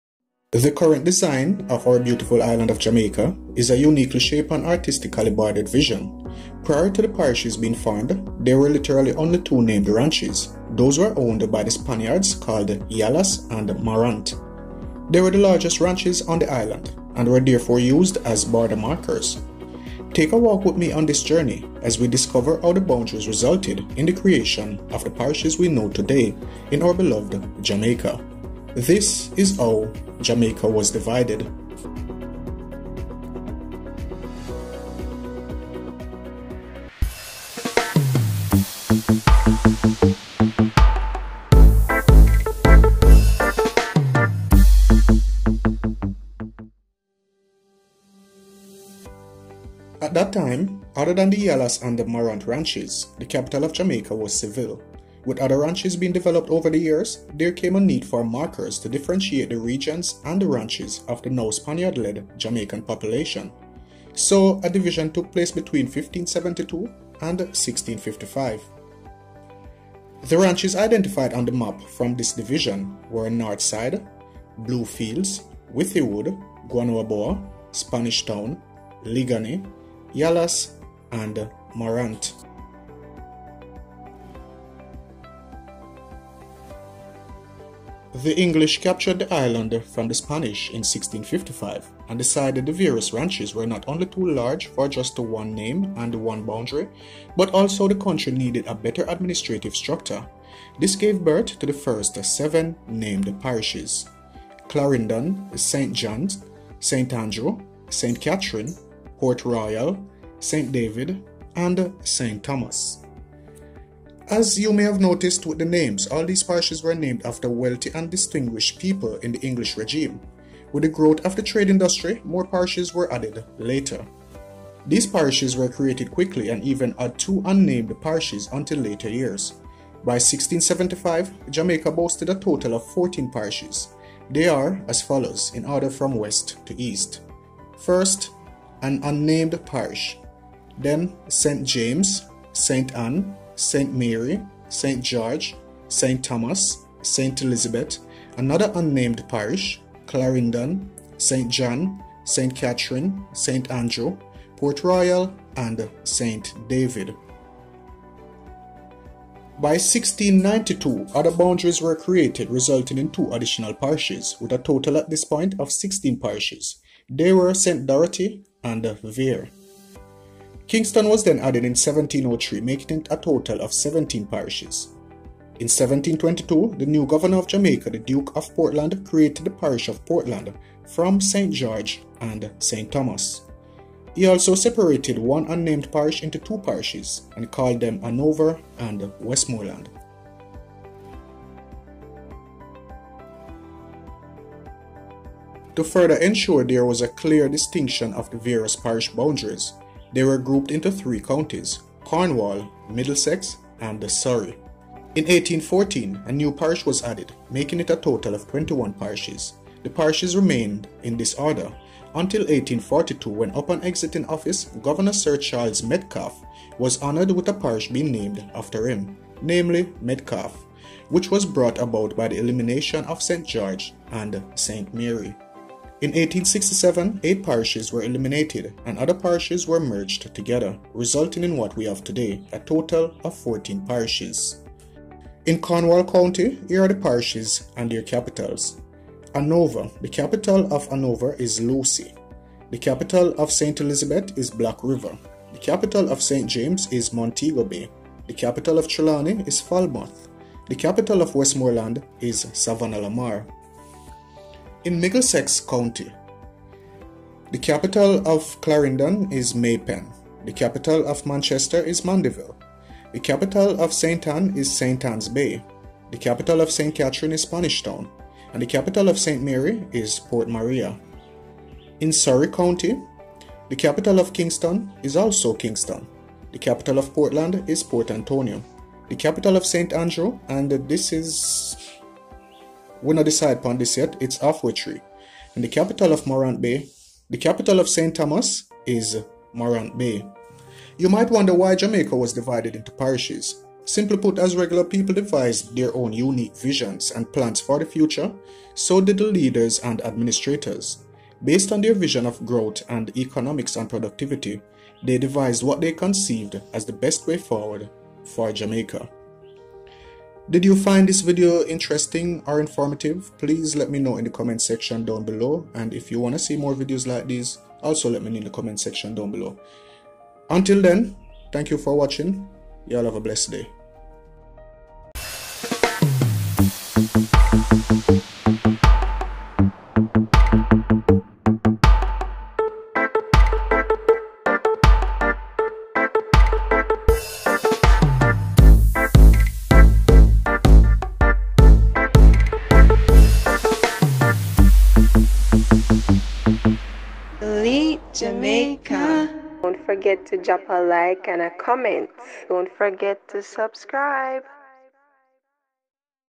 This is how Jamaica was DIVIDED (The Borders of Jamaica)
This Elite Jamaica video explains how the country of Jamaica was divided into counties and parishes.

This is how JAMAICA was divided.